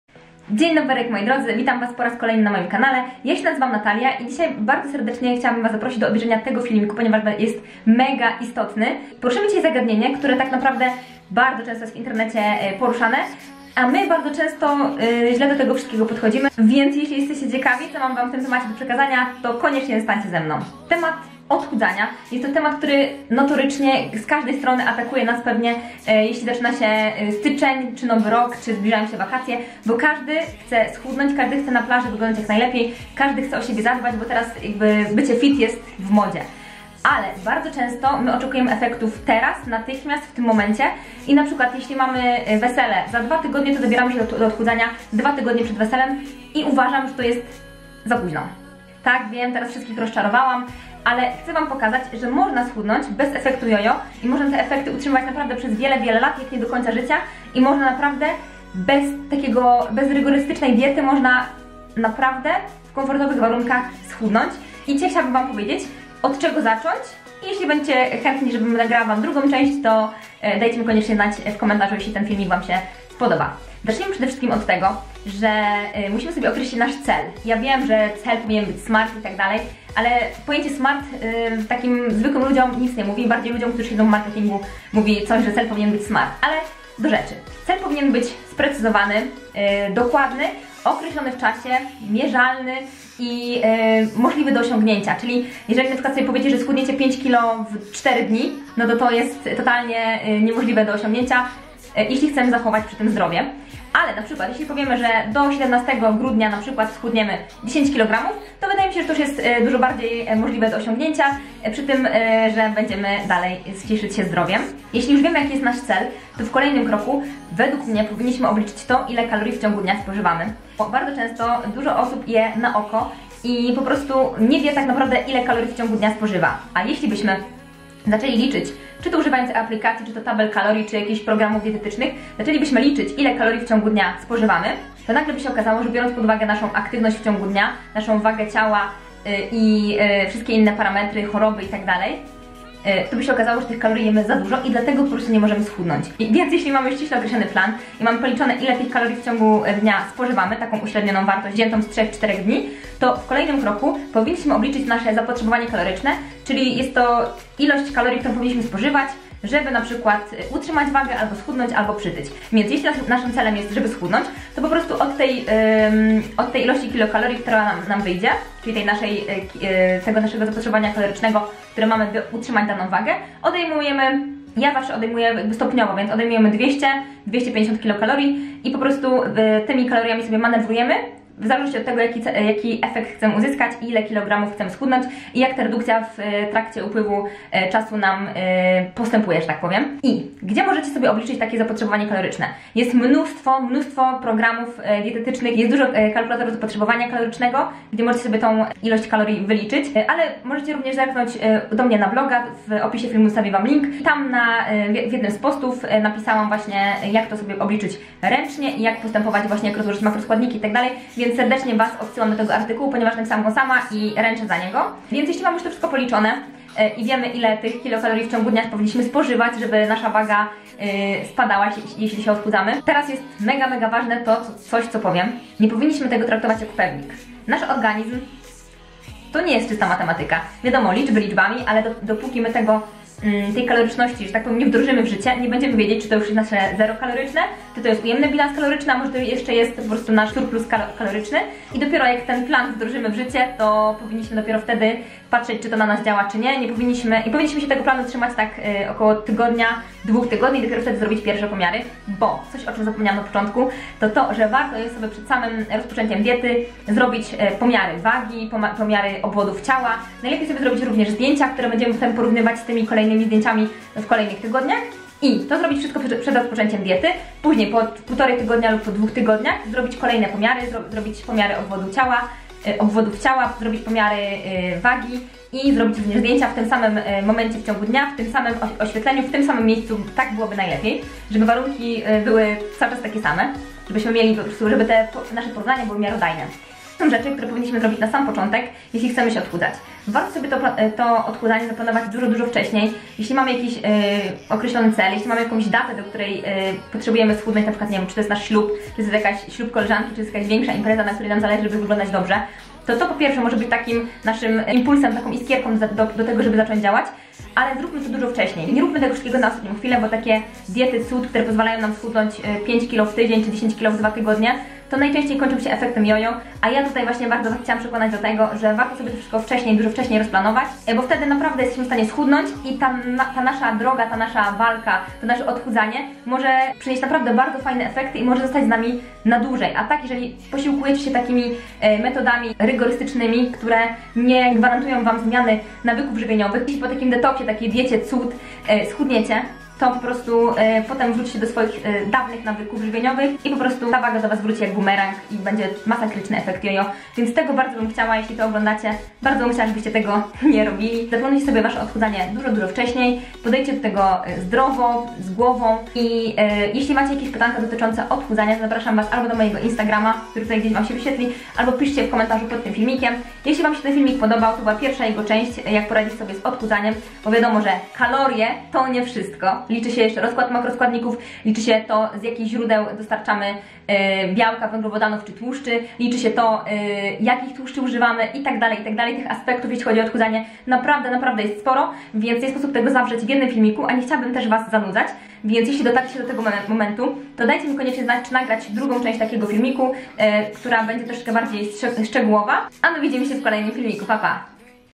SCHUDNIJ 4 kg BEZ EFEKTU JOJO *sprawdzone tipy* DIES NATALIS
Czy odchudzanie bez efektu jojo w ogóle jest możliwe?
W dzisiejszym filmiku pokażę Wam od czego zacząć w ogóle odchudzanie i jak sprawić by zrzucone kilogramy nie wróciły jak bumerang po zakończonej diecie!

Dajcie znać czy chcecie drugą część? :)

GDZIE MNIE ZNAJDZIECIE?